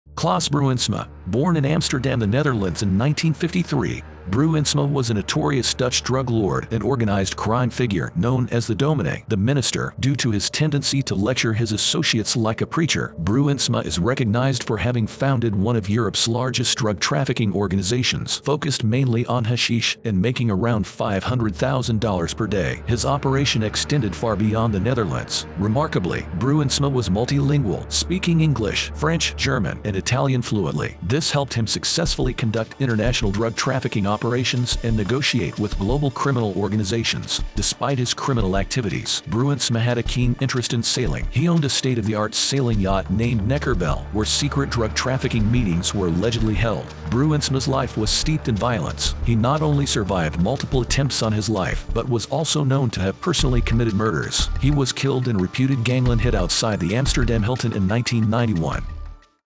Klaas Bruinsma: Rise and Fall of a Dutch Drug Lord
Join us on an immersive journey into the life of Klaas Bruinsma, the legendary Dutch drug lord. From his unprecedented rise to his sudden fall, witness the real story behind the headlines.